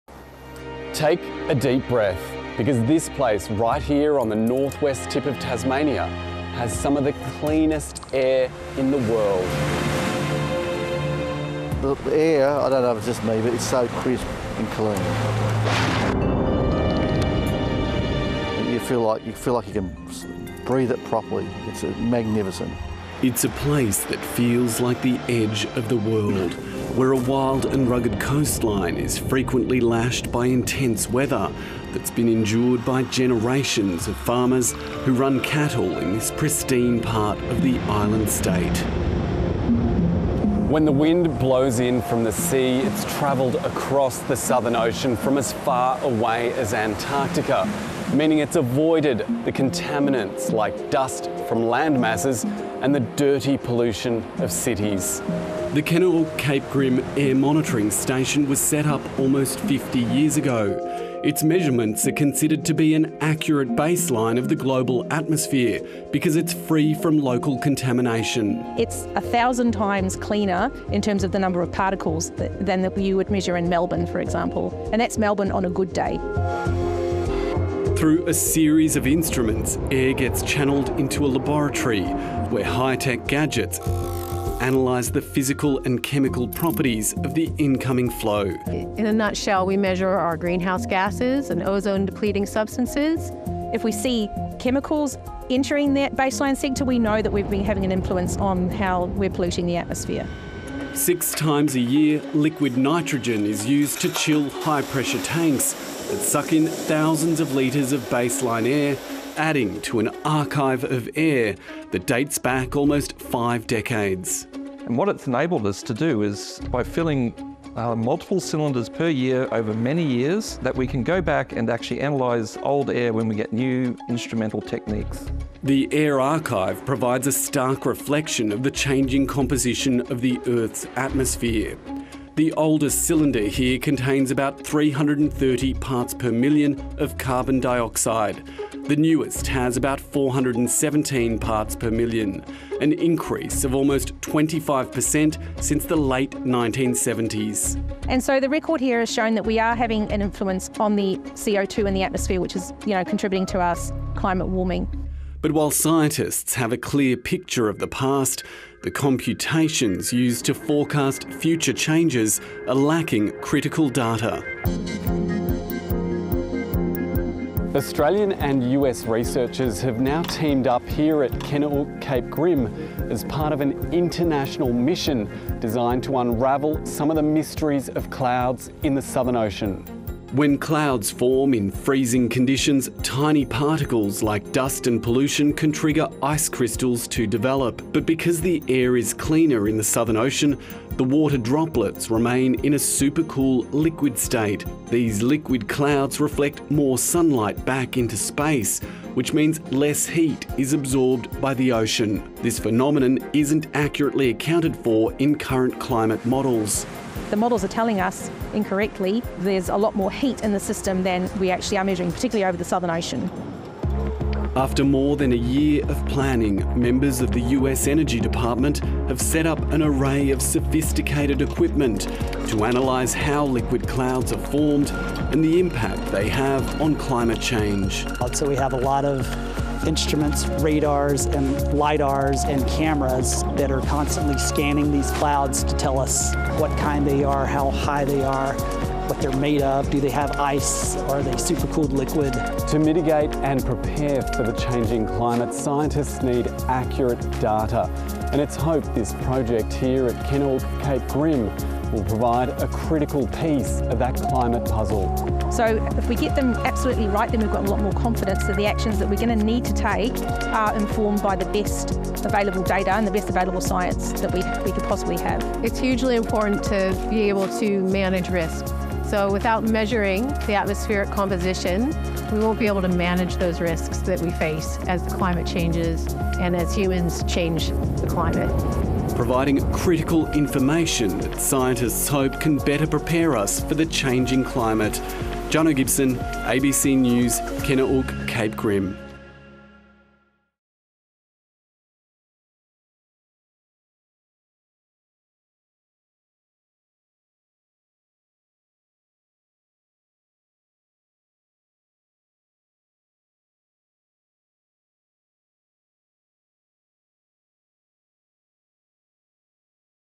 Cape Grim has world's cleanest air, and it's helping solve a climate puzzle | ABC News
For nearly five decades, the Kennaook/Cape Grim air-monitoring station on Tasmania's north-west tip has been pivotal in tracking human-induced changes to the global atmosphere.

Australian and US scientists have teamed up to analyse "super-cooled" liquid clouds over the Southern Ocean, which are skewing climate models because of a lack of data.

The "CAPE-K" project will run until late next year, with the aim of improving our ability to forecast long-term changes to the climate.